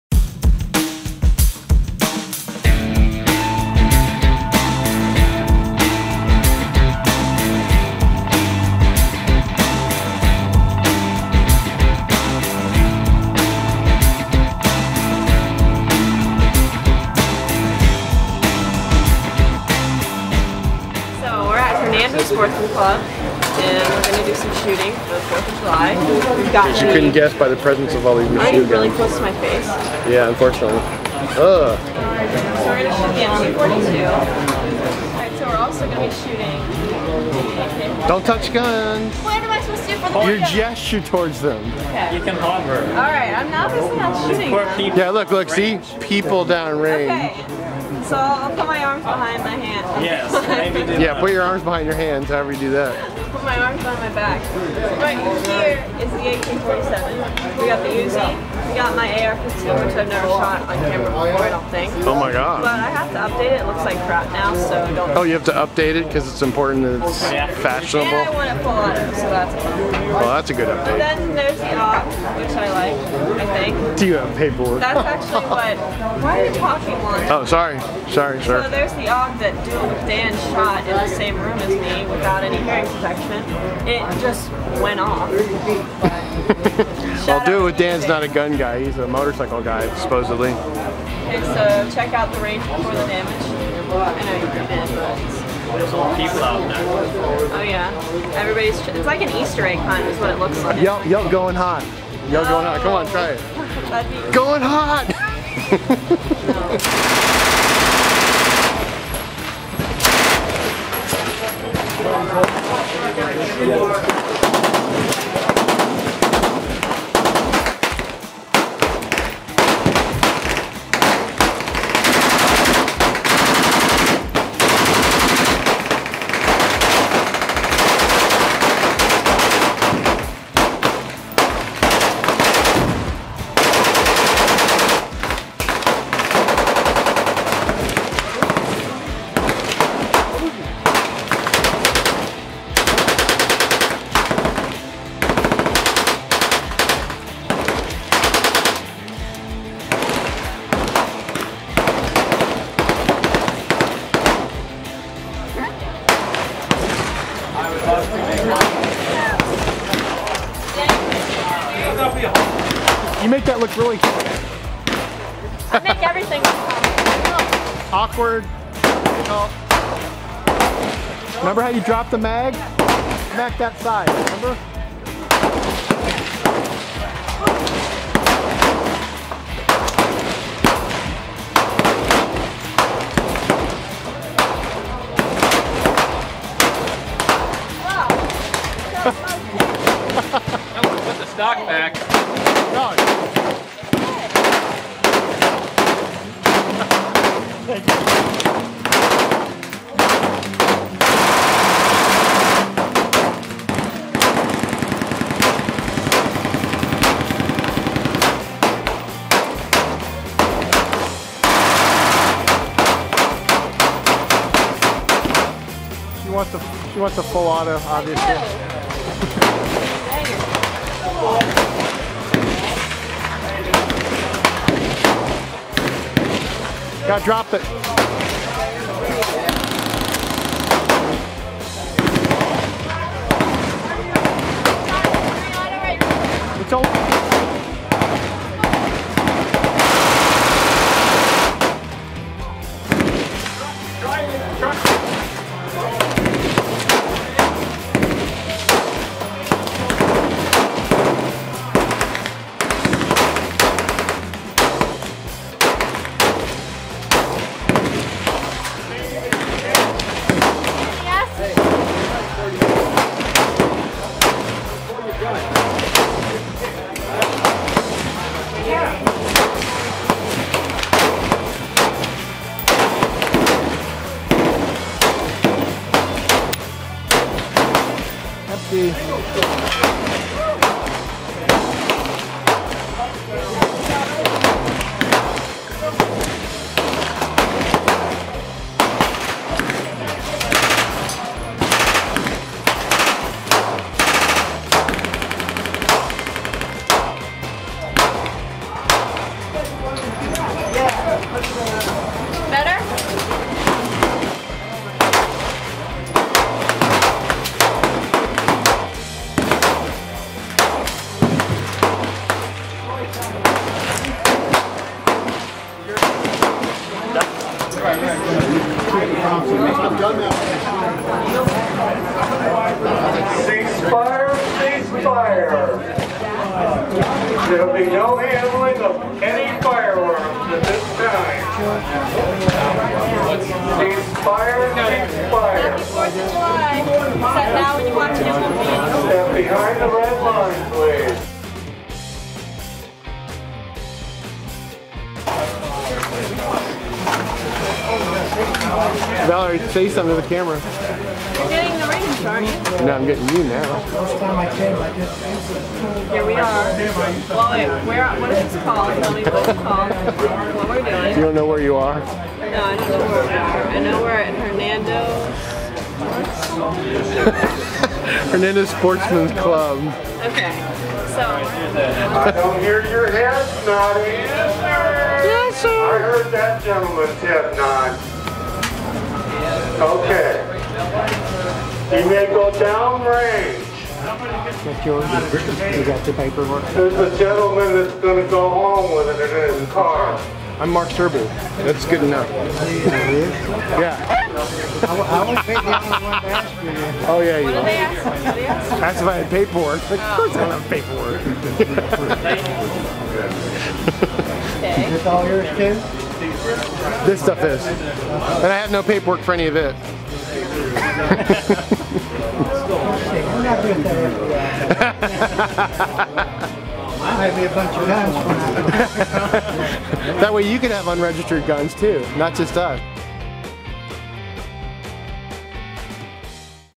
MG 42 Madness
Didn't do fireworks on the fourth of July, but I did the next best thing if not THE best thing an American can do to celebrate... :-P Hope you all had a wonderful day as well!